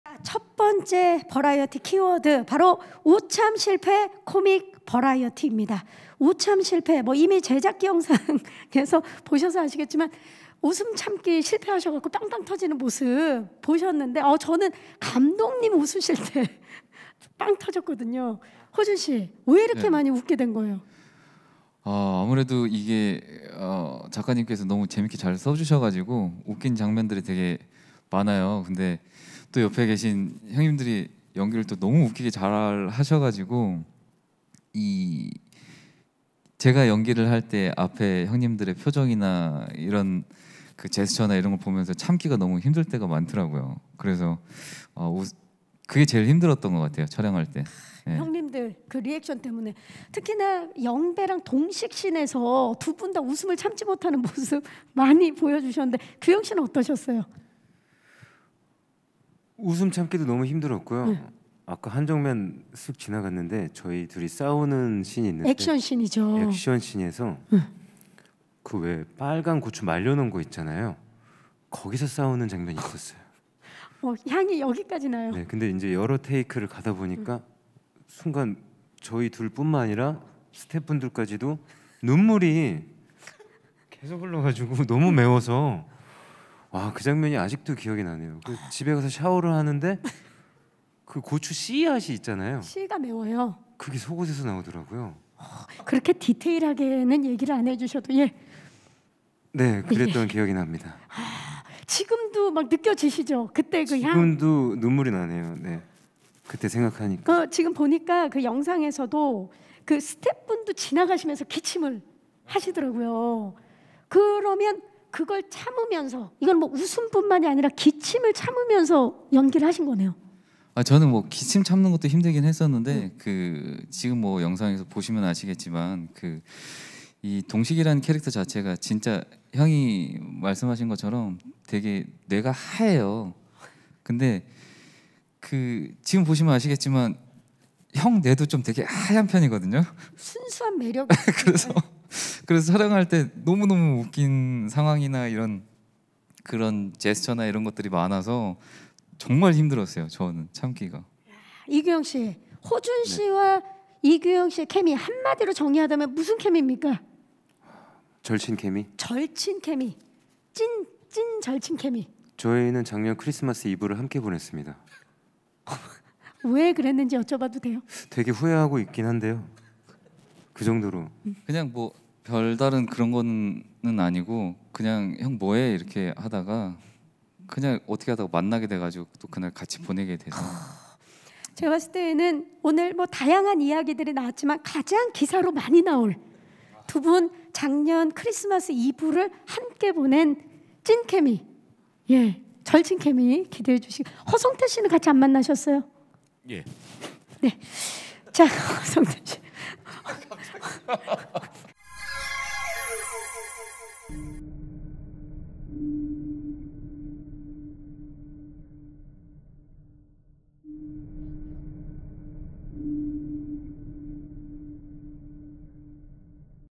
[ENG] 손호준(Son Ho Jun)x이규형, 케미 터졌던 ‘고추씨 사건’ @ 영화 ‘스텔라’ 제작보고회 (허성태 Heo Sung Tae)ㅣTongTongCulture
3월 17일 영화 '스텔라'의 제작보고회가 열렸다. 이날 행사에는 배우 손호준, 이규형, 허성태, 권수경 감독 등이 참석했다.

The press conference of the movie 'Stellar' was held on the 17th of March. In the conference actor Son Ho Jun, Lee Kyu Hyung, Heo Sung Tae and director Kwon Sookyung were present.

📍Korean pop culture channel of Yonhap News Agency.